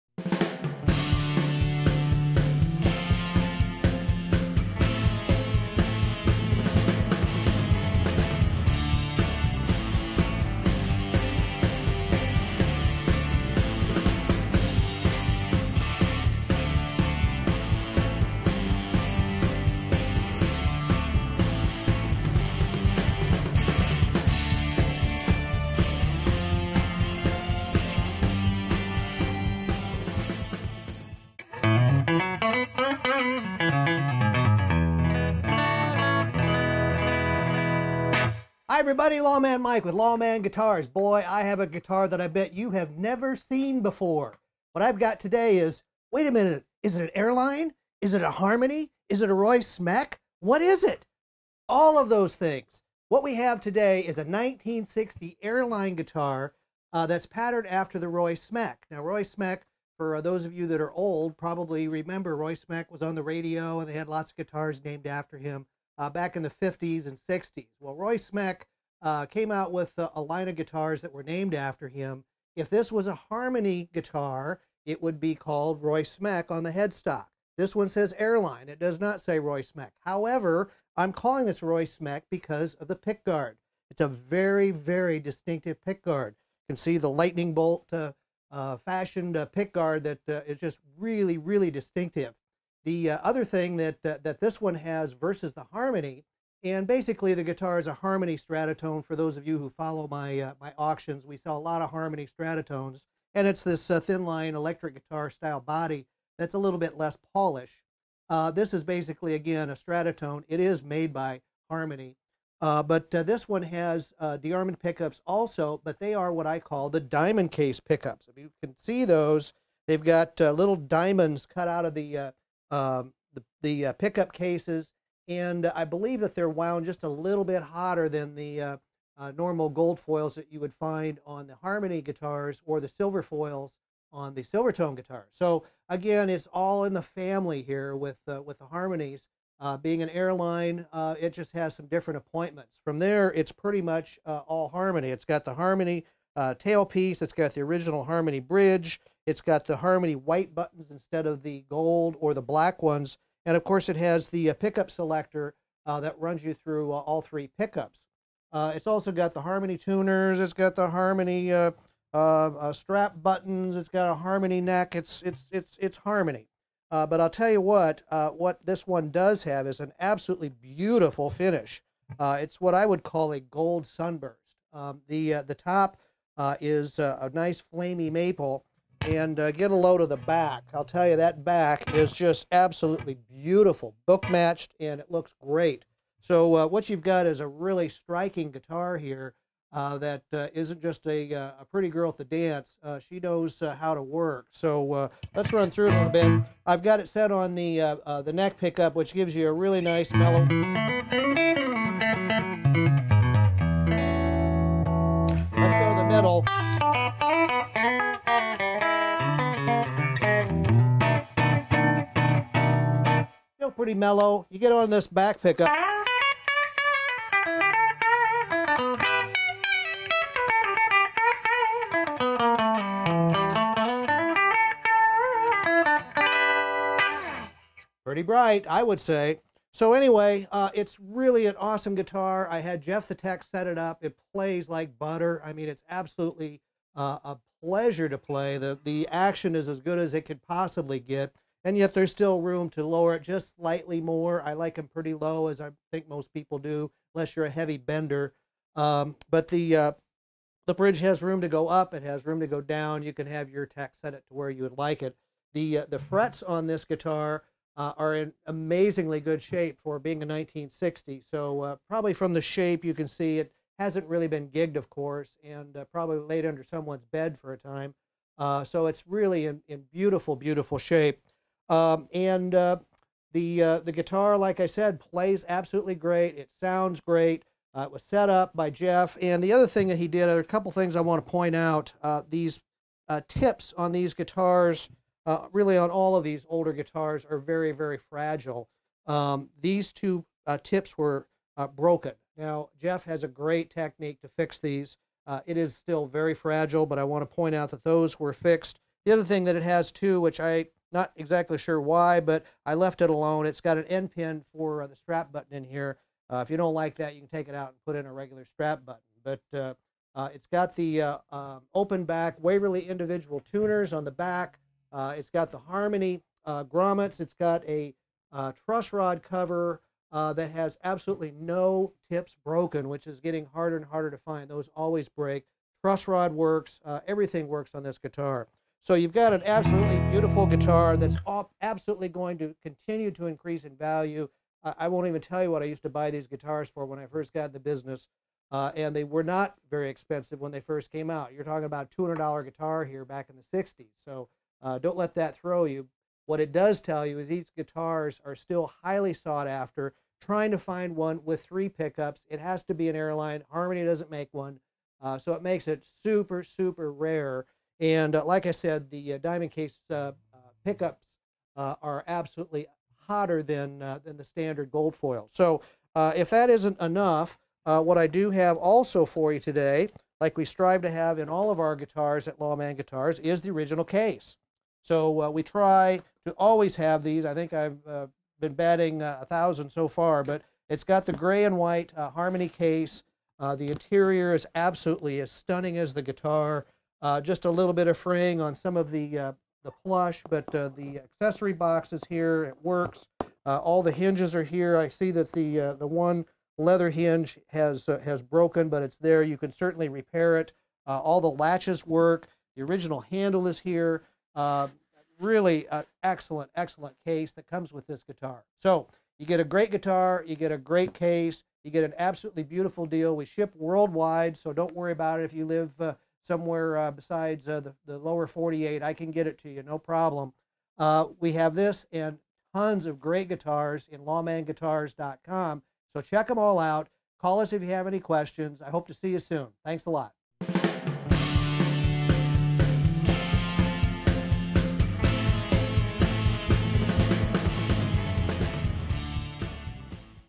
Used Guitars for Sale: 1960 Airline 3 Pickup Guitar with case, Used Guitars Sale
1960 Airline 3 Pickup Roy Smeck Electric Guitar and Original Case

I always have Cool Guitars in my shop but sometimes one shows up that is beyond cool. How about a 1960 Airline Roy Smeck.

Harmony built this guitar and they would have branded it a Roy Smeck with a raised Roy Smeck Logo.  Airline didn't stop at two pickups like the Harmony. They had to have THREE! The Pickups in this Guitar are Extraordinary. I call these DeArmond Pickups the Diamond Case pickups from the design in the top. They are usually wound a little Hotter than the Standard Gold or Silverfoil DeArmond Pickups. These are no exception. They Sound Warm when you want it and Scream when Needed. If you have not heard these pickups before, Be Prepared to Love Them.

The Guitar is All Original. There were two pickguard tips that had broken off but  fortunately they were still with the guitar. Jeff the Tech was able to glue and secure them. Hard to see his repairs. They are still fragile so be careful when taking the pickguard off.

I snapped a pic of the interior of the guitar to date it. See pics. It has the Harmony date stamp of 1960. I had Jeff set this guitar up with Awesome Easy Playing low action. You will Love how this Guitar Feels. It's Harmony built and the body would also be made like this for the Harmony Stratotones and the Silvertone Jupiters. That will give you a clue to its size. Hollow inside so it's a Very Resonate Guitar. All Original Parts from the White Knobs and Pickup Selector Switch and Truss Rod cover. The Frets are in Great shape and with normal playing will last the life of most guitar players. It has an end pin in the strap button spot. It looks original, so I left it alone. That's the only suspect of something not correct from the factory. A very easy fix.

As you know, we strive to have original cases with our guitars. Nothing adds value to Vintage Guitars like the Original Case. This Guitar is No Exception. Original Harmony Gray and White Softshell  Case with awesome Clean Red Plush Interior. All Hinges, Latches and Handle are intact and functioning. A great case for a Great Guitar.

This Unique Instrument will only continue to increase in value. This model is UBER RARE. I have only seen a couple in my lifetime and this is the first I have had. You will not be disappointed in this guitar. Grab it while you can.

Topic: Used Guitars for Sale

This video was produced by Tiller Productions, Des Moines, Iowa